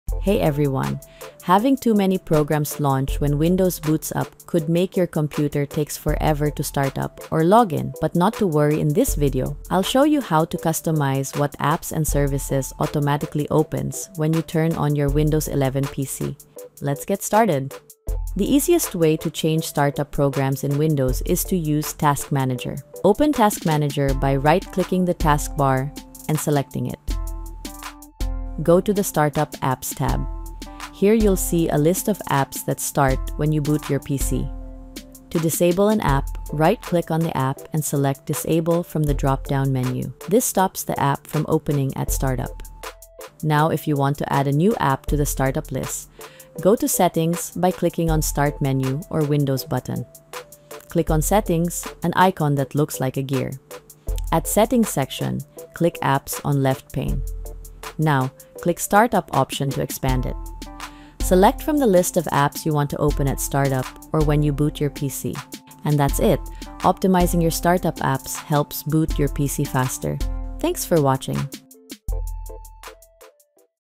How to Add or Remove Startup Apps on Windows
Learn how to customize which programs launch when you boot your Windows PC. This tutorial shows you how to use Task Manager's Startup tab to easily enable, disable or delete apps from auto-starting in Windows 11, 10 or 7.

Speed up your boot time by optimizing unnecessary startup programs with these simple steps. We'll also cover adding apps to run at login using the Startup folder.